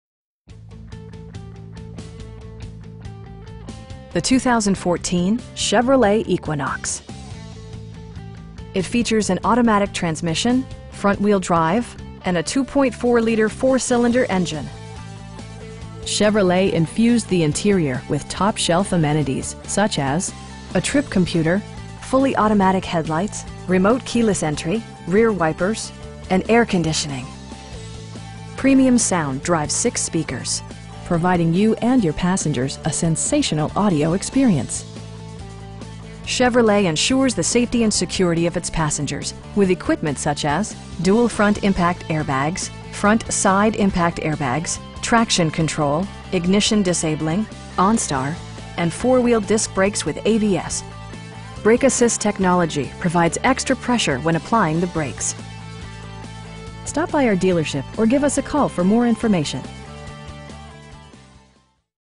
Hank Graff Davison - Chevrolet Equinox LS - For Sale Davison MI
Chevrolet Equinox LS

 The 2014 Chevrolet Equinox. It features an automatic transmission, front-wheel drive, and a 2.4 liter 4 cylinder engine. Chevrolet infused the interior with top shelf amenities, such as: a trip computer, fully automatic headlights, tilt and telescoping steering wheel, power door mirrors, remote keyless entry, rear wipers, and air conditioning. Premium sound drives 6 speakers, providing you and your passengers a sensational audio experience. Chevrolet ensures the safety and security of its passengers with equipment such as: dual front impact airbags, front SIDE impact airbags, traction control, ignition disabling, onStar, and 4 wheel disc brakes with ABS. Brake assist technology provides extra pressure when applying the brakes. You will have a pleasant shopping experience that is fun, informative, and never high pressured. Stop by our dealership or give us a call for more information.